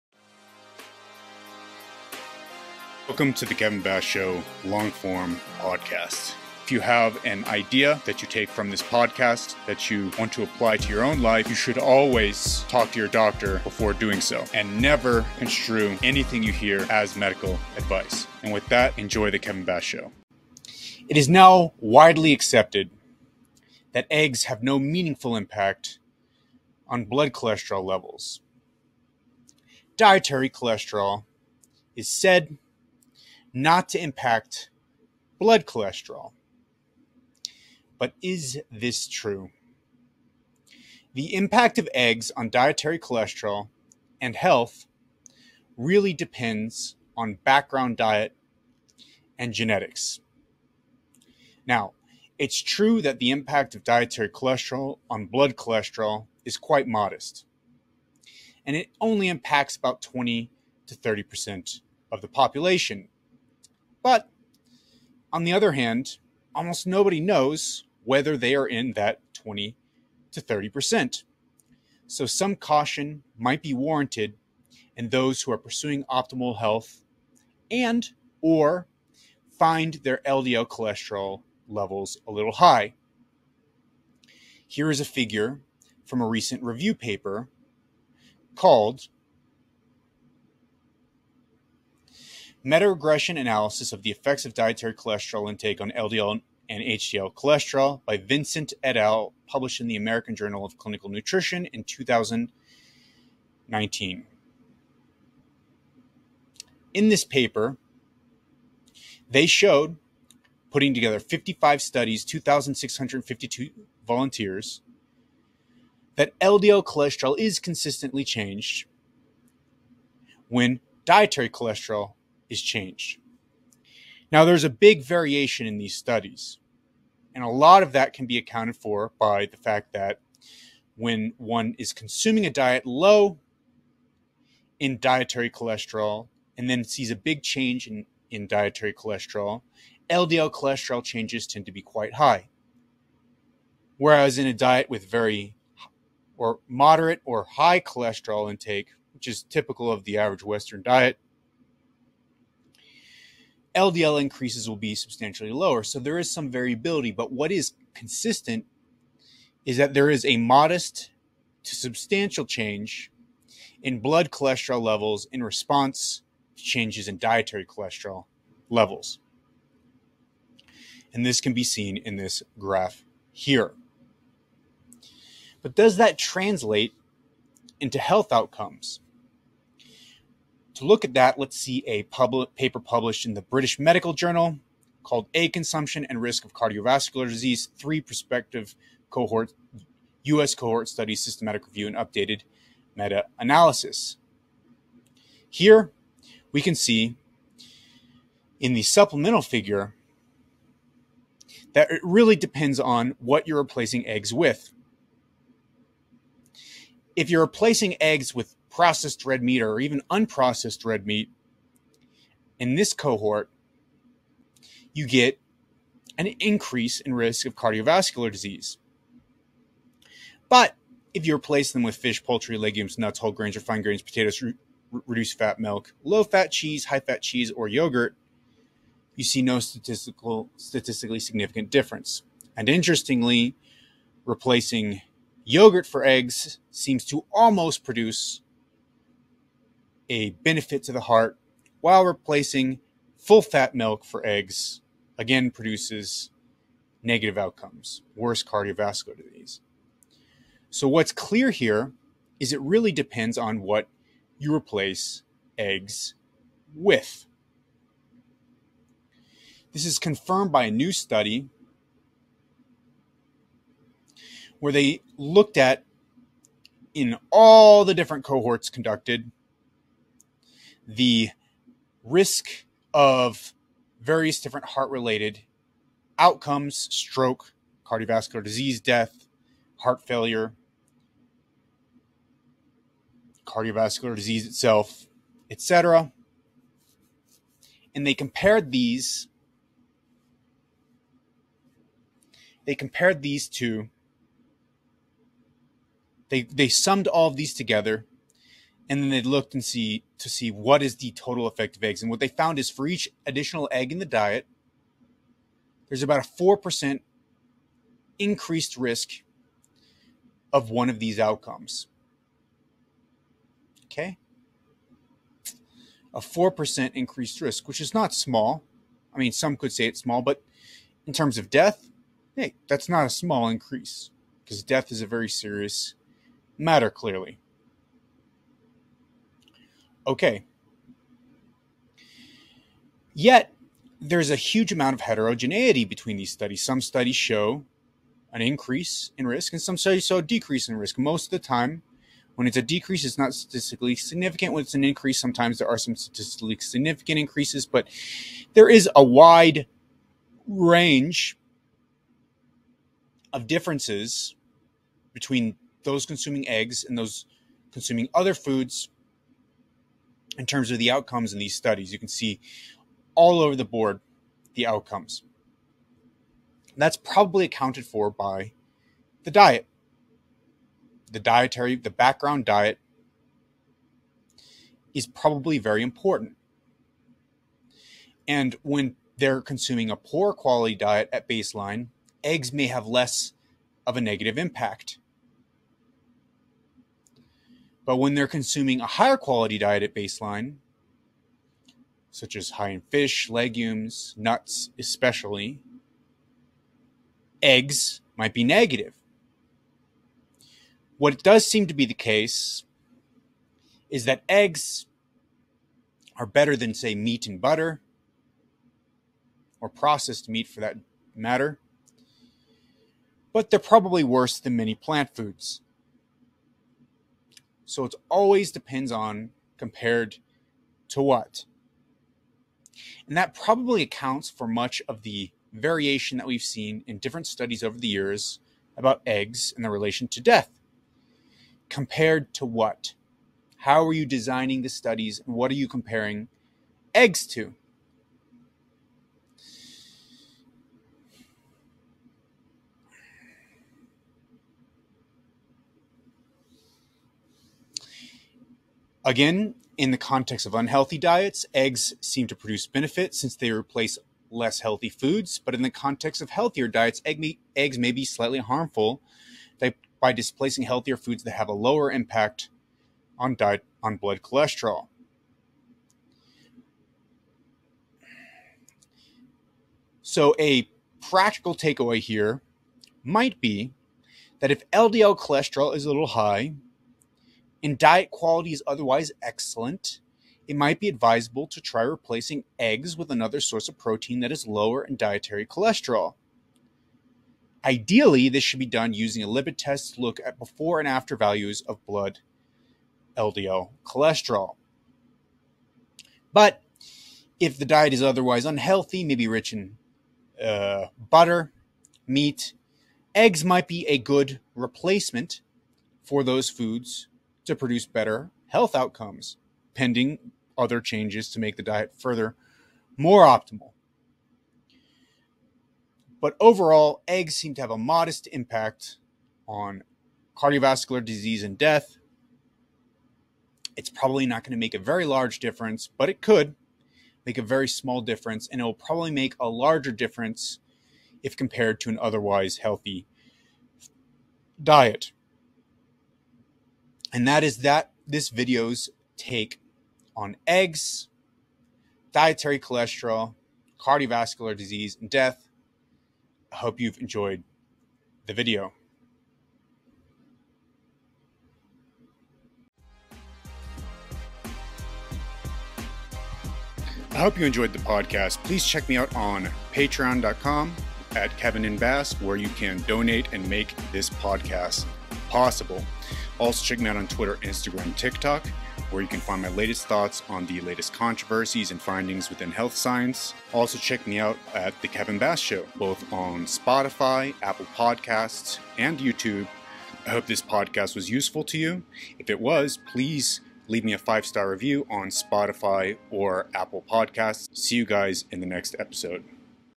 Eggs, cholesterol, and health
It is now widely accepted that eggs have no meaningful impact on blood cholesterol levels. Dietary cholesterol is said not to impact blood cholesterol.

But is this true?

The impact of eggs on dietary cholesterol—and health—really depends on background diet and genetics.

Now, it’s true that the impact of dietary cholesterol on blood cholesterol is quite modest [1], and it only impacts about 20-30% of the population [2]. But, on the other hand, almost nobody knows whether they are in that 20-30%, so some caution might be warranted in those who are pursuing optimal health and find their LDL cholesterol levels a little high.

In the context of unhealthy diets, eggs actually seem to produce benefit, since they replace less healthy foods.

But in the context of healthier diets, eggs may be slightly harmful, by displacing healthier foods that have a lower impact on dietary cholesterol.

Overall, the impact of eggs on health seems quite heterogeneous and may depend on the baseline diet: more healthy diets may seem more harm if eggs are incorporated, and vice versa [3,4].

If LDL cholesterol is a little high, and diet quality is otherwise excellent, it might be advisable to try replacing the eggs with another source of protein that is lower in dietary cholesterol.

Ideally, this should be done using a lipid test to look at before and after values of blood LDL cholesterol.

PMIDs
[1] 30596814
[2] 24466502
[3] 32132002
[4] 35360933

For more, find me at:
PODCAST The Kevin Bass Show